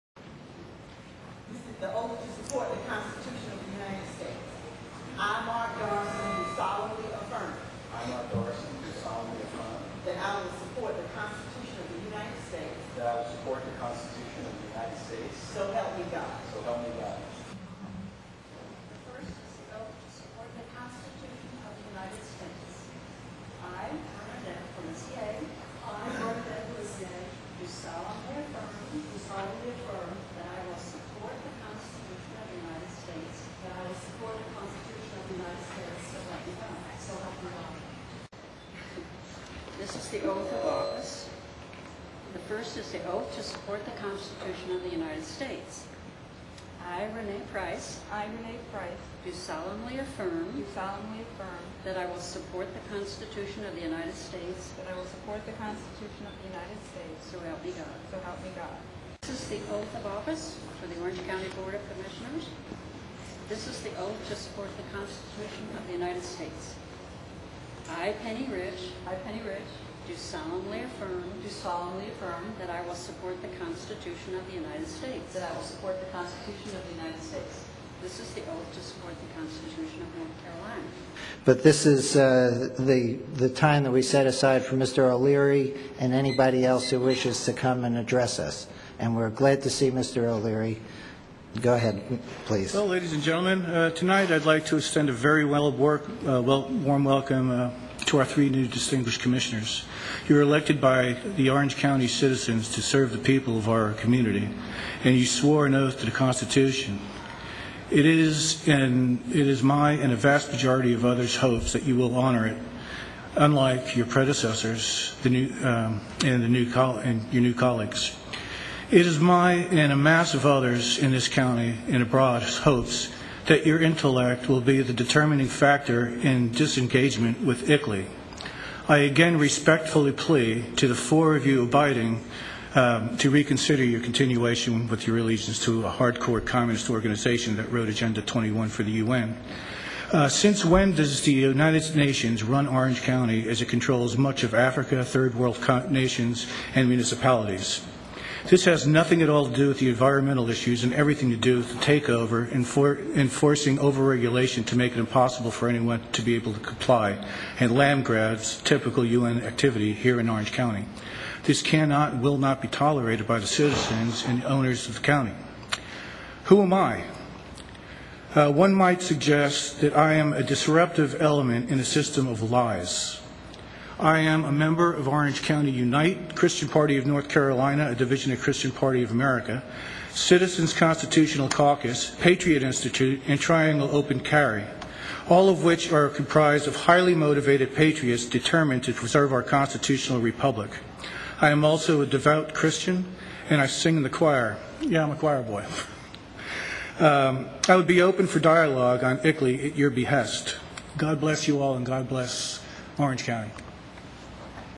Swearing In New Commissioners & Welcoming Them In Hope Of Disengagement Of ICLEI
12/3/12 The title says it all. Swearing in the Orange County, NC Commissioners.